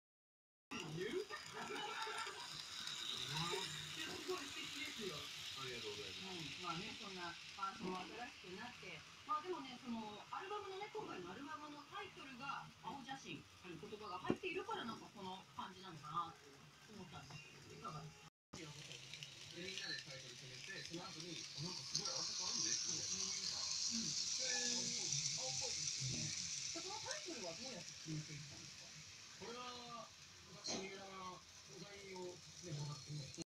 [鉄道模型] 名鉄3307F 室内灯装備化+@
GM製 名鉄3307Fに、室内灯LCを装備させました。
部屋を暗くしても存在感を放ちます。
GM製車両でよく言われるボディ透けは見受けられませんが、3101Fは乗務員室部分が透けてしまいます。

余談
2017年度設備投資が発表されました。
新造は3310F.3311F.3170F.3171F.3172Fの模様です。
リニュパノ化は今年度も3編成改造で、
2213F増備はなく、1131Fは暫く生き残りそうですね。